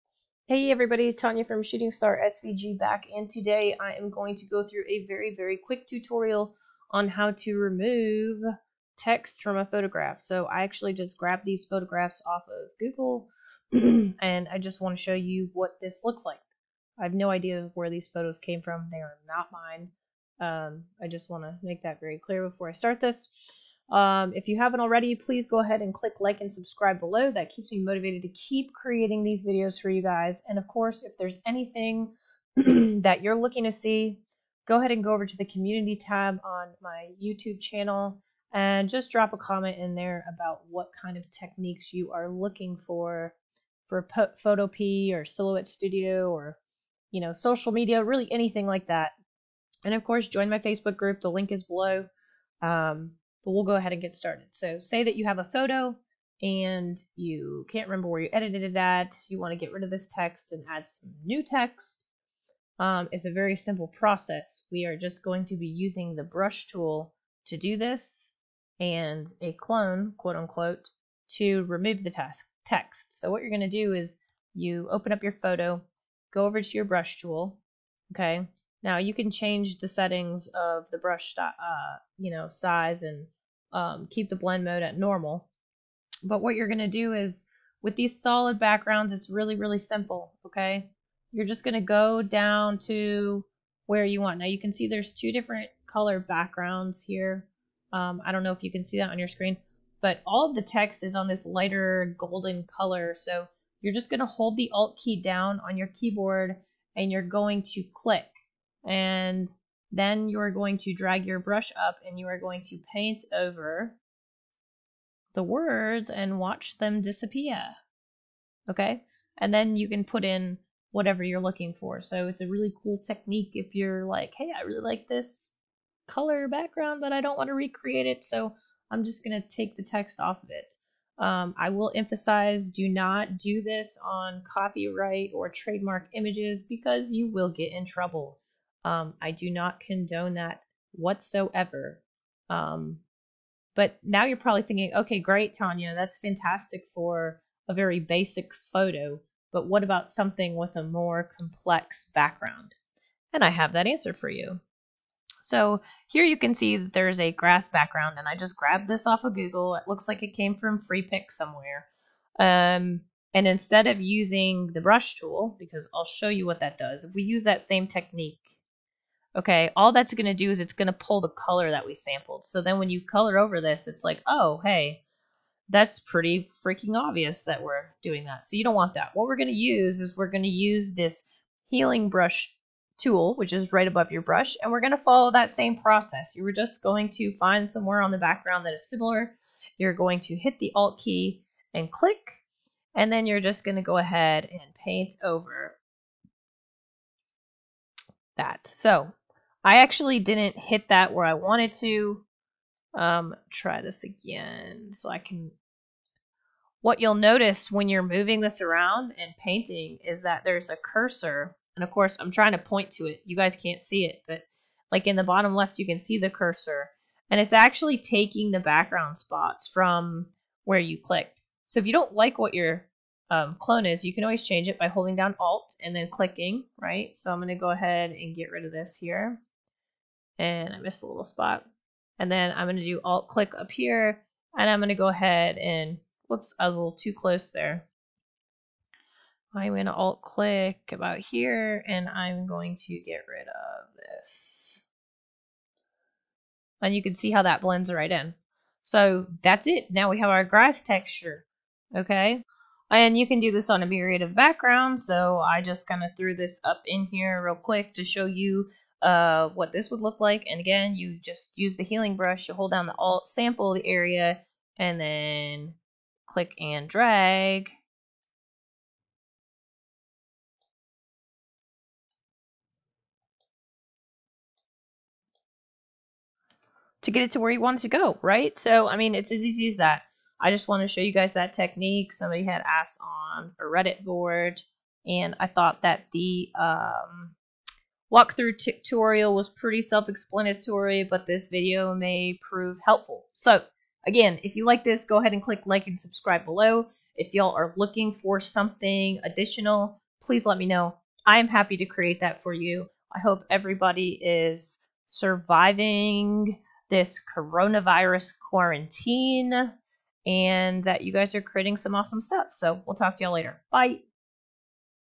Remove Text from a Background in Photopea
Learn how to remove text from a background in Photopea with this quick video tutorial. If you're looking to remove text on a photo you can use these same techniques.